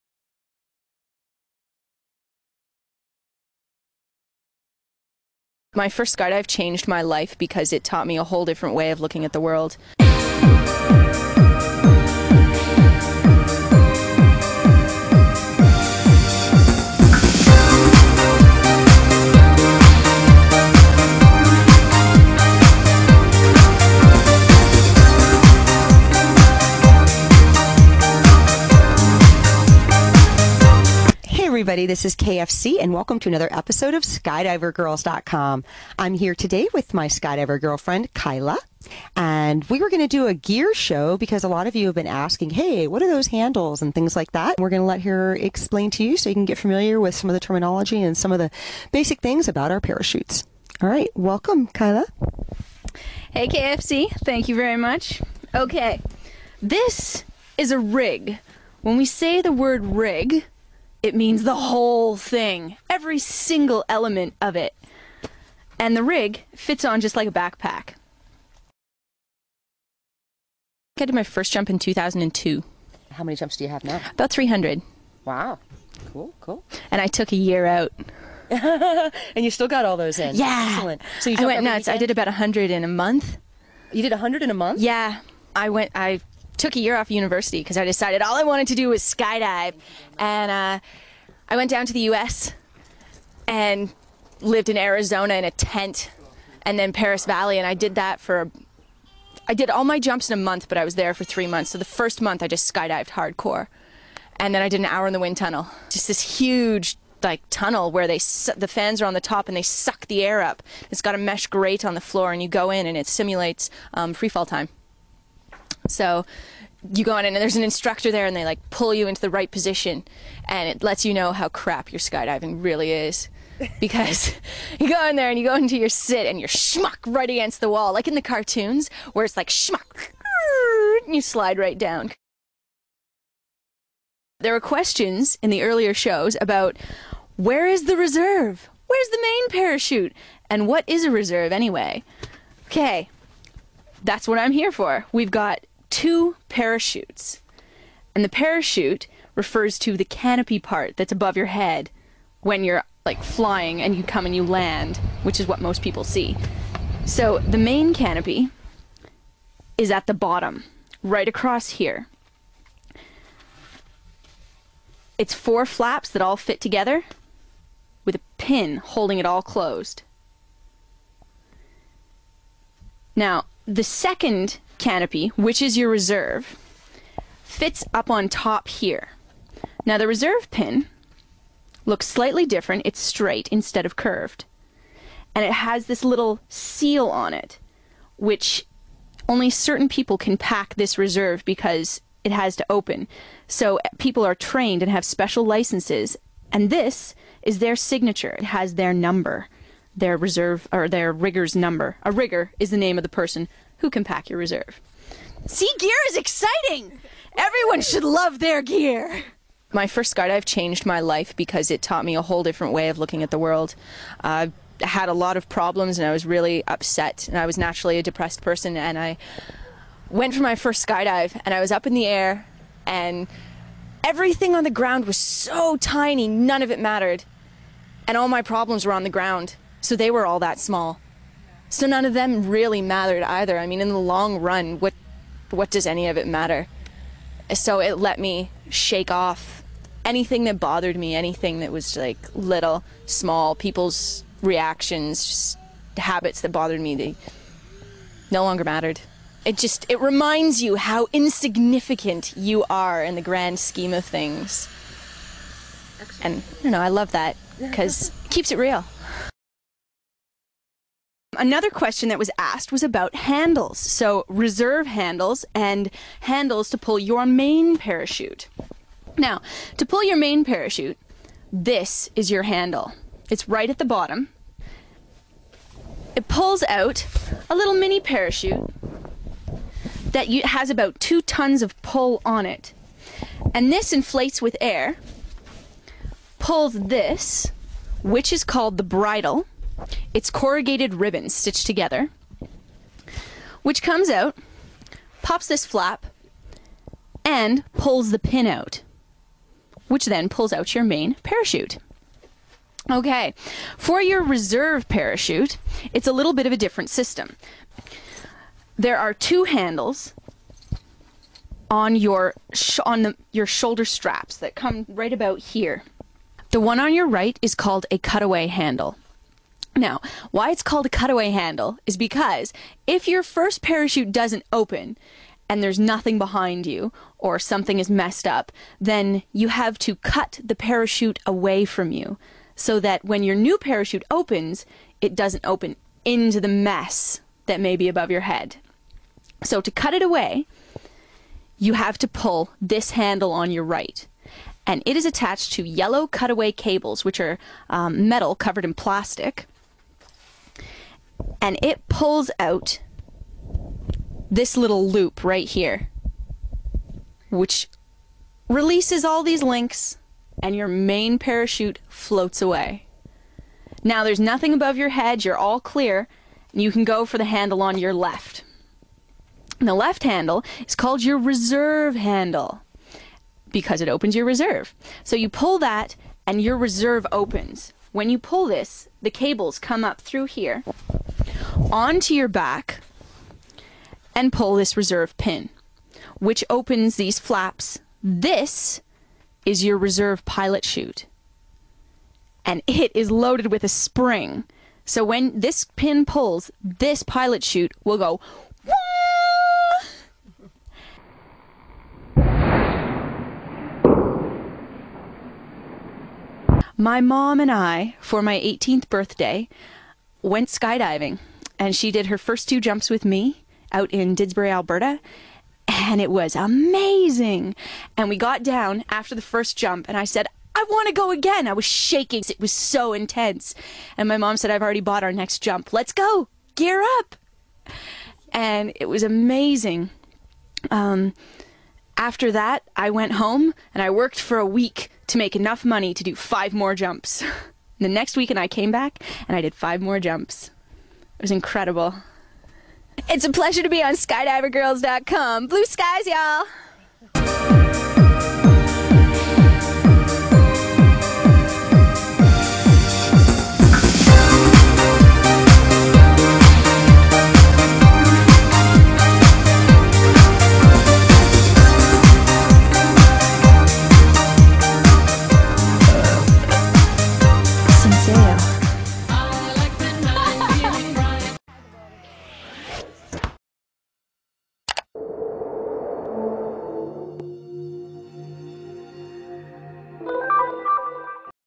SKYDIVERGIRLS-004 (Kyla - SKYDIVERGIRL & Professional Packer
Kyla - SKYDIVERGIRL & Professional Packer of Parachutes Episode 004 (time=07:51)  Who packs your parachute?  It's a common question and in many cases, someone like Kyla is the answer.  She is a pro and shares some basic information about what all that parachute stuff is anyway.   Music by Bill Tustain